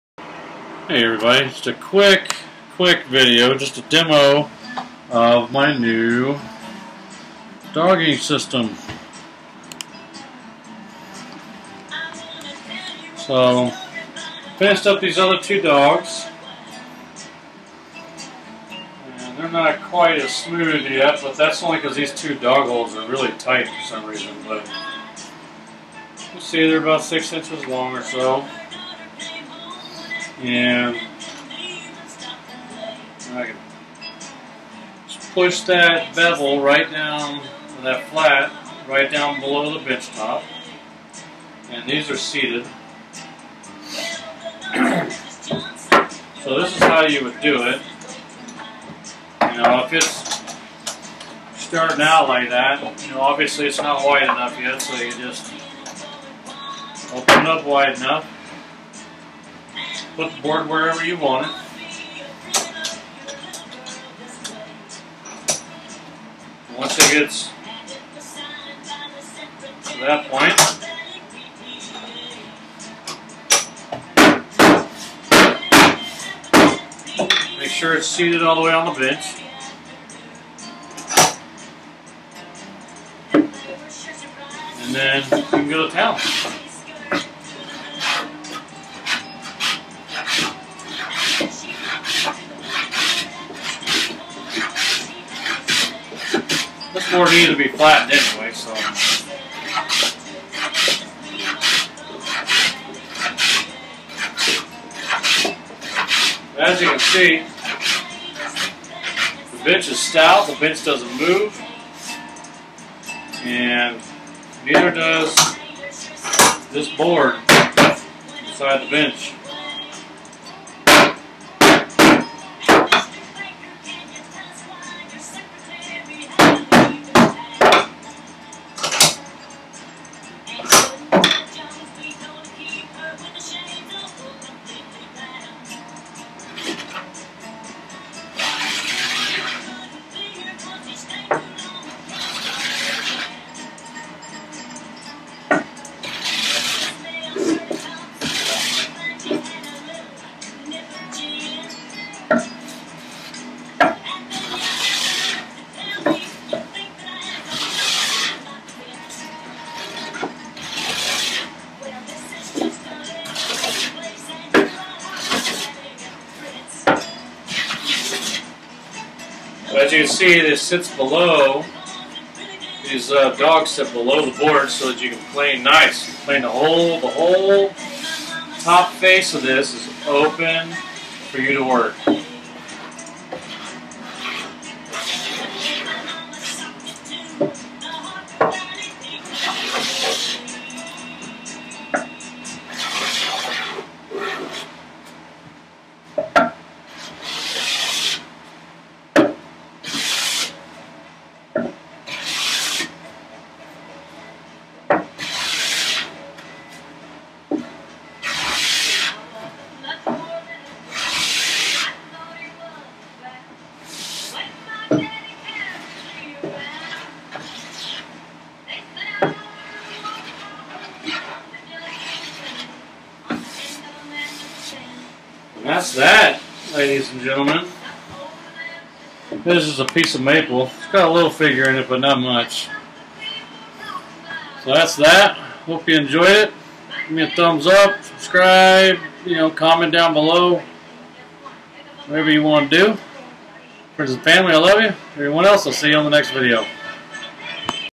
Cordless Carpenter workbench build #77 - Bench dogging system demo
Finished up the other two dogs so here's a quick demo of WHY i did this and as you can see or imagine just how essential it is to have a way to hold your work solidly like this.